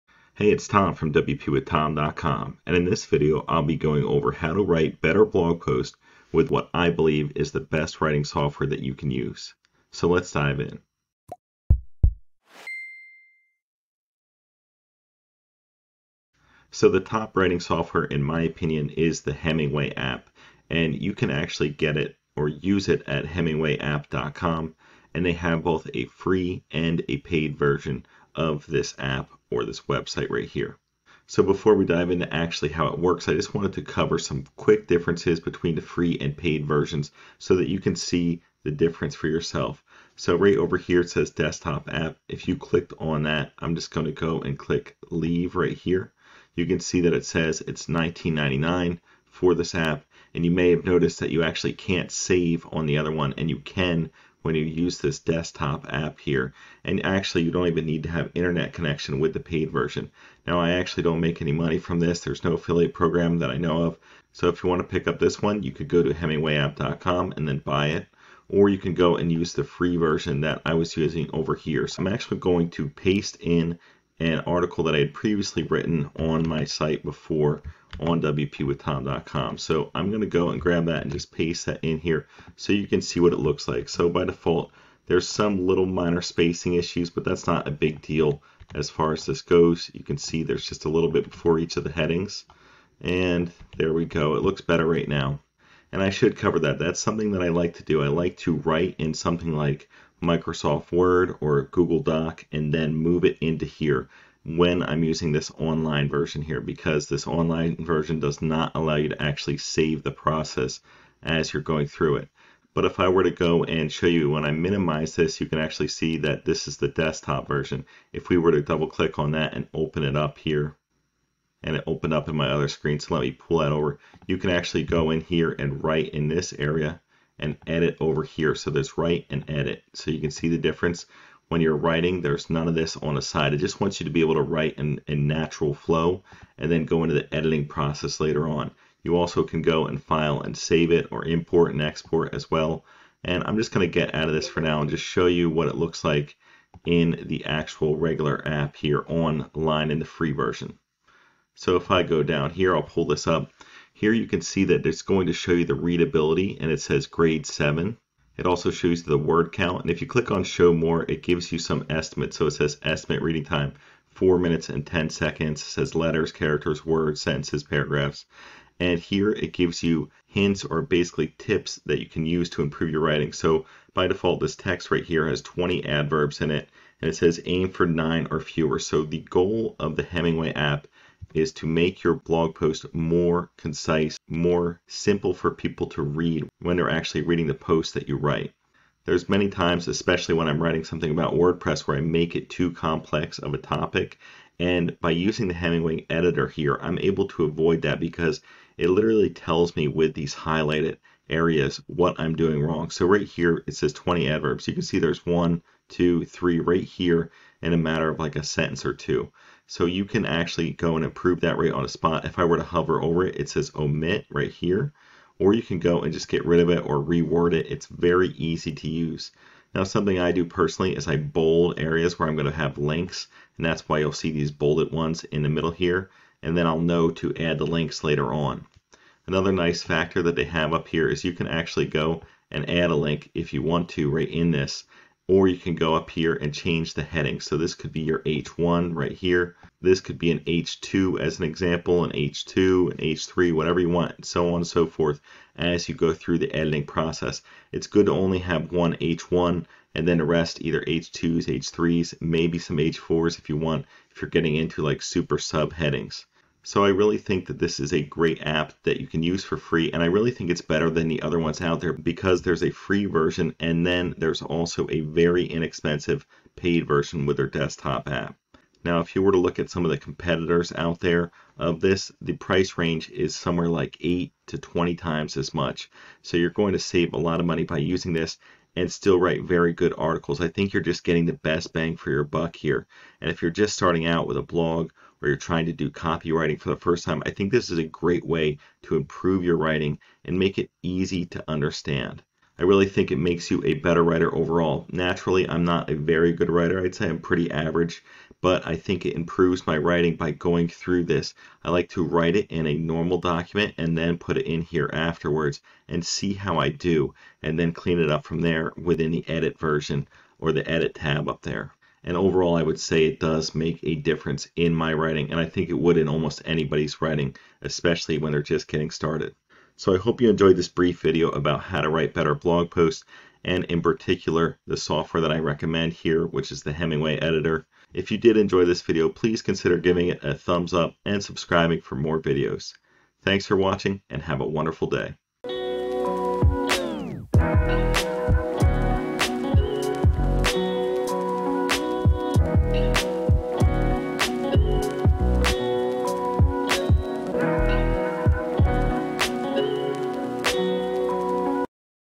How to Write Better Blog Posts | Best Writing Software
In this video I cover how to write better blog posts with what I believe is the best writing software out  there. The Hemingway tool helps you learn how to write better. It makes you write concise, easy to understand sentences. I hope you enjoy this video and give it a try for yourself!

Best Hosts:
NameHero:

WordPress Builders:
Elementor:

WordPress Theme:
Astra:

SEO:
SEMRush: